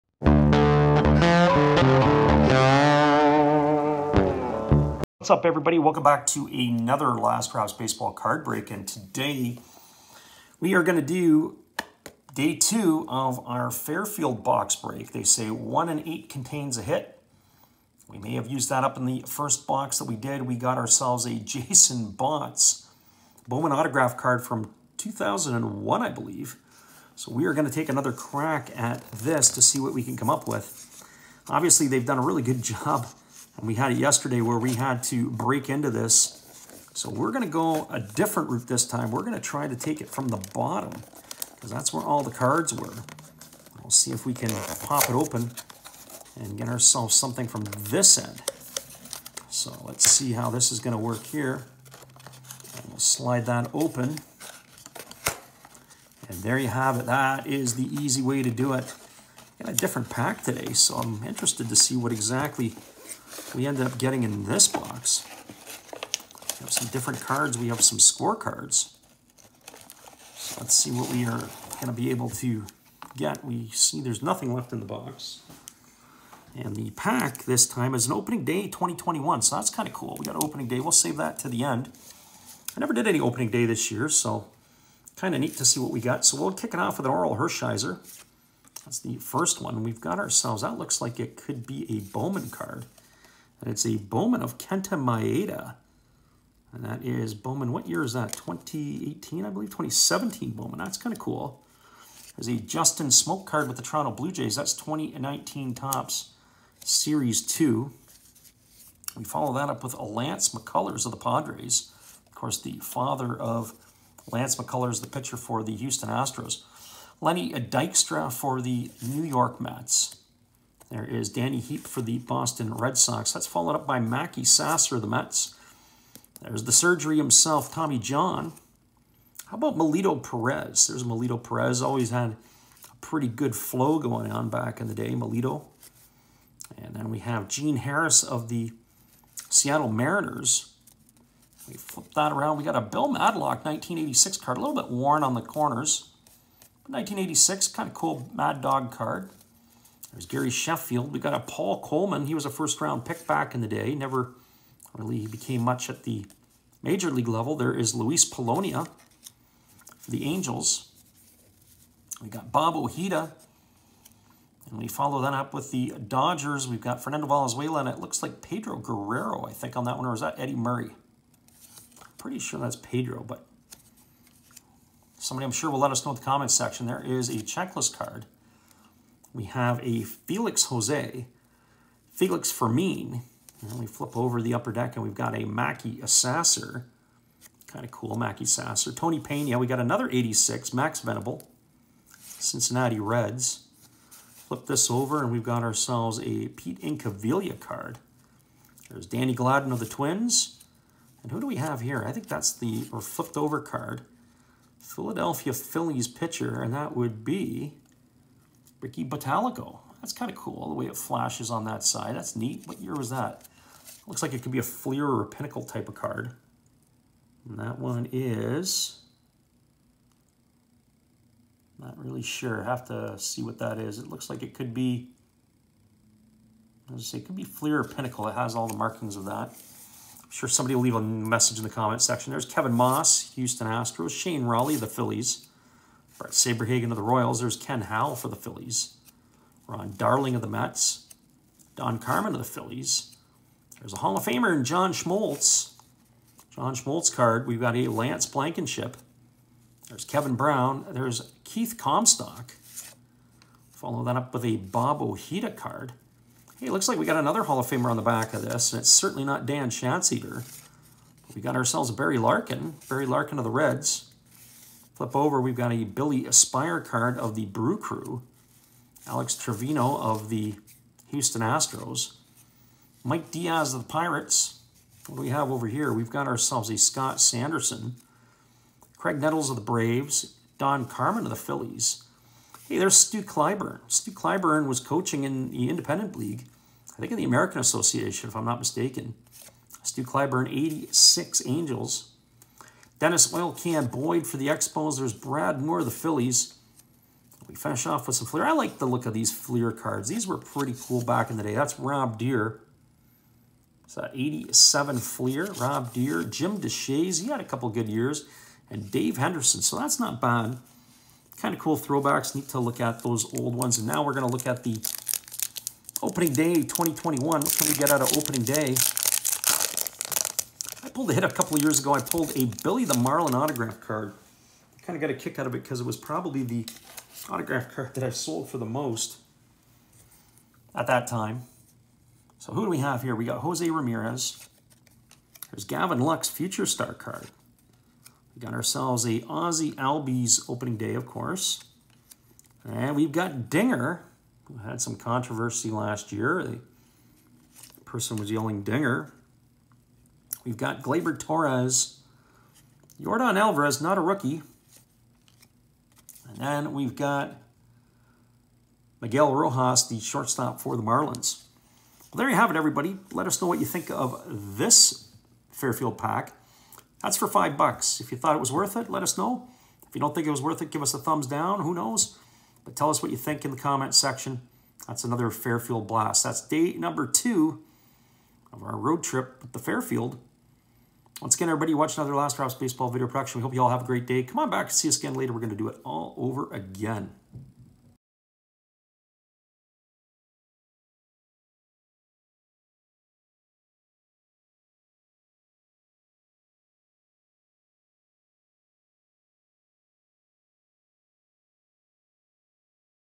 Fairfield Baseball Box Break - Shooting Blanks?
Here comes day two of our Fairfield Baseball Box Break.  We did this just over a week ago in Bellingham, WA, and what we found was that baseball cards were tough to find in the USA!  Baseball cards in the State of Washington were tough to find, and these were some of the few products we were able to get!!    There wasn't anything spectacular, but we were glad to pull at Topps Opening Day pack from the box.  We didn't get anything up here, so it was a first for us!!   Check out the video and let us know what you think.